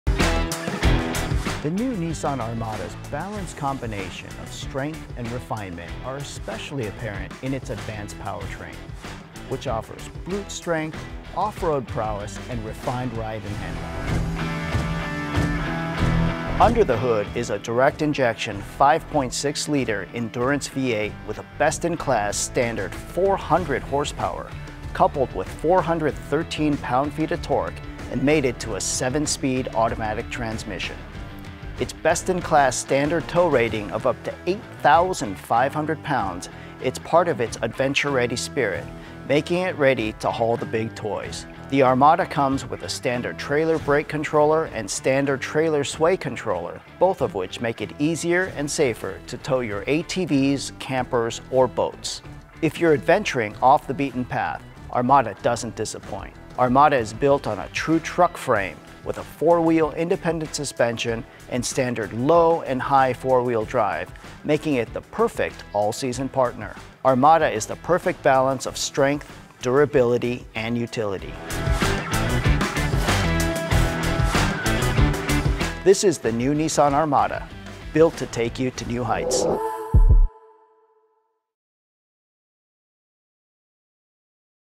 2021 Nissan Armada | Capability
The new Nissan Armada’s balanced combination of strength and refinement are especially apparent in its advanced powertrain. Offering brute strength, off-road prowess and refined ride & handling.

Under the hood, is a direct injection 5.6-litre Endurance V8 engine with a best-in-class standard 400 HP coupled with 413 lb-ft of torque. Made into a 7-speed automatic transmission, the Armada has a best-in-class standard 8,500-lb towing capacity, reflecting its adventure ready spirit.

The Armada comes with a standard trailer brake controller and standard trailer sway controller, both of which make it easier and safer to tow your gear!

Adventuring off the beaten path? The Armada doesn’t disappoint. The 2021 Armada is built on a true truck frame with 4-wheel independent suspension and standard low and high 4-wheel drive.

The Armada is the perfect balance of strength, durability and utility. This is the new Nissan Armada, built to take you to new heights.

To learn more about the 2021 Nissan Armada, visit us

To get the latest updates from Nissan Canada, follow our official social channels: